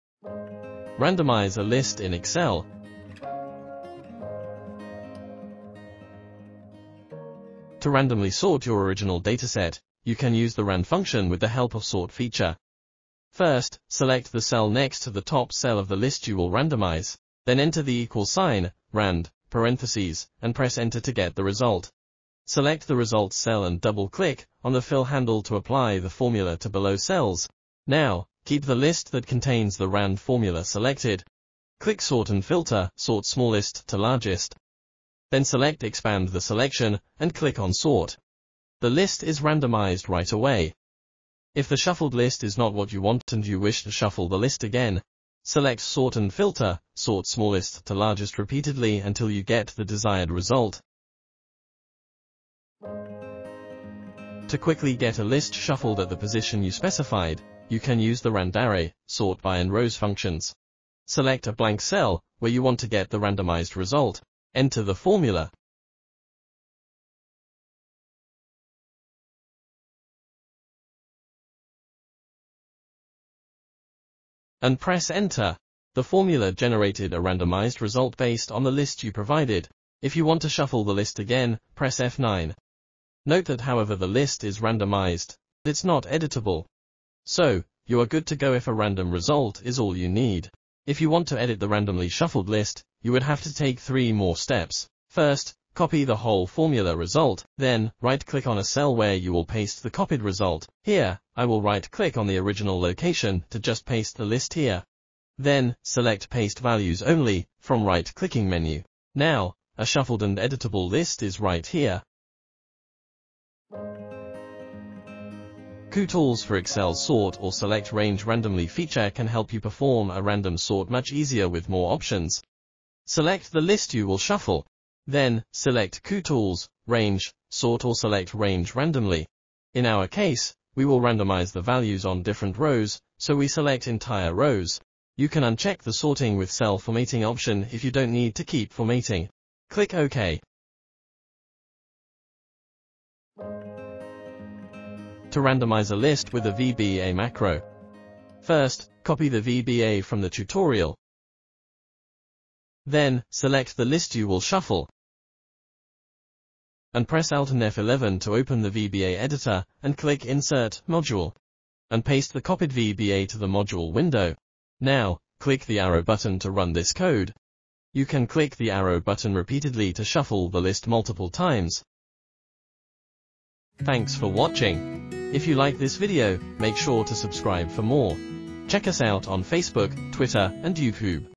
Randomize a list in Excel (Easy step-by-step tutorial)
Performing random sort is a quick and easy way to ensure that you end up with all cases turning up with equal probability. In this tutorial, we will talk about how to easily randomize, or shuffle a list in Excel in few steps.

00:00 Intro
00:06 Randomize a list with RAND function
00:55 Get list shuffled at the specified position (only available for Excel 365 and Excel 2021 users)
01:55 Randomize a list with Kutools in 2 clicks with more options
02:25 Randomize a list with VBA macro